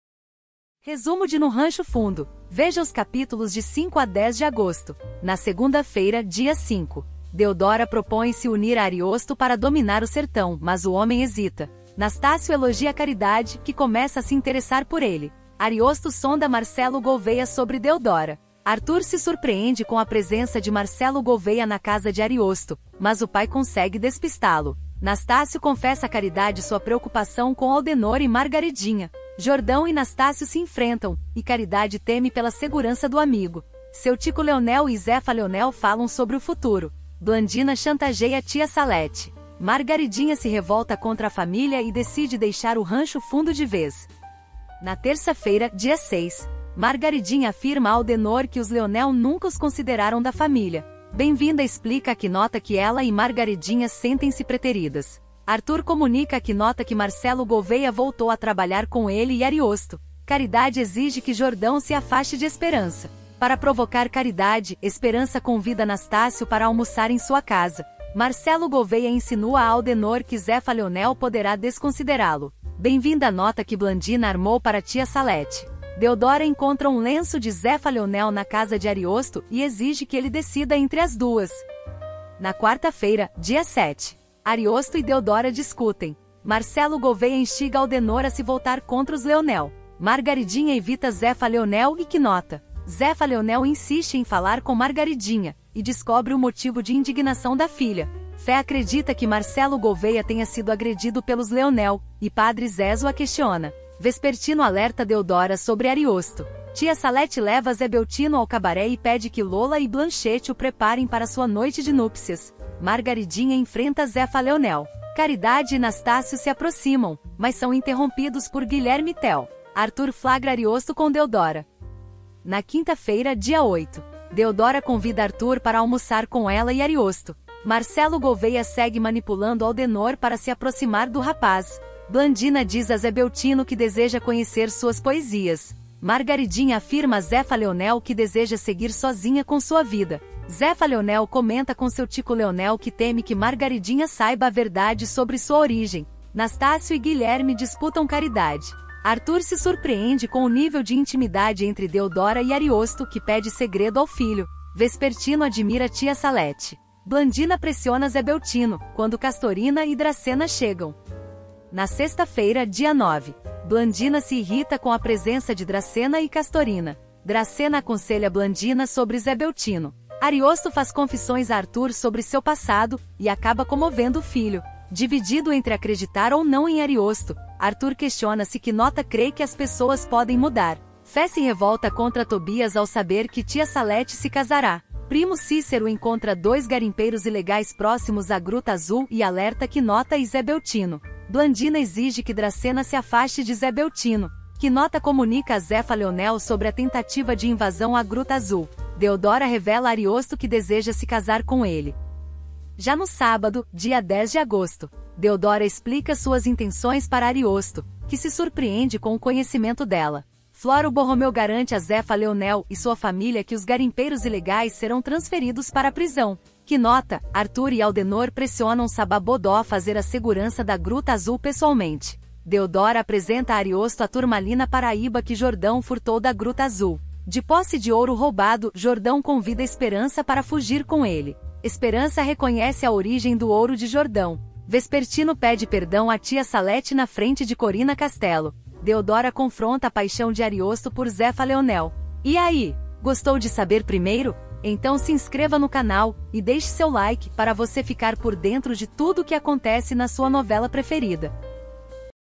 NO RANCHO FUNDO – RESUMO DESTA SEMANA – CAPITULOS DE 5 A 10 DE AGOSTO - VEJA TUDO QUE VAI ROLAR
NO RANCHO FUNDO – RESUMO DESTA SEMANA – CAPITULOS DE 5 A 10 DE AGOSTO - NOVO CASAL E MAIS UMA TURMALINA AZUL: SAIBA O QUE VOCÊ NÃO PODE PERDER ESTA SEMANA